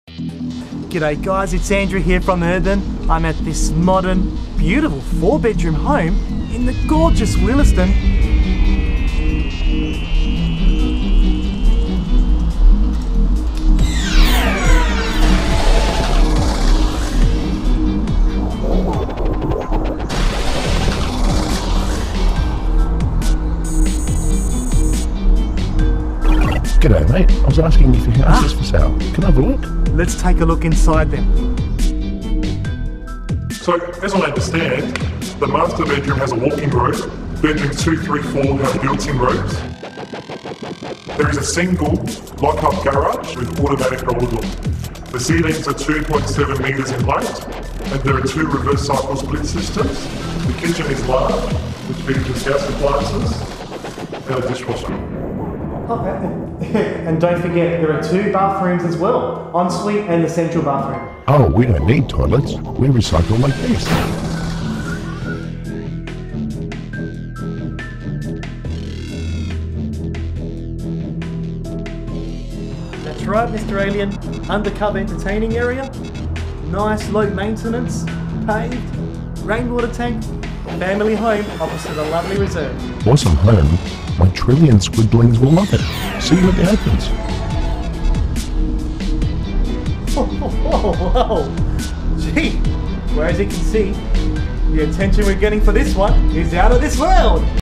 Neat modern home in Willaston SA 5118 presented by Andrew Kyriacou from Urban Residential
Real Estate Adelaide
Andrew Kyriacou presents this neat modern home in the suburb of Willaston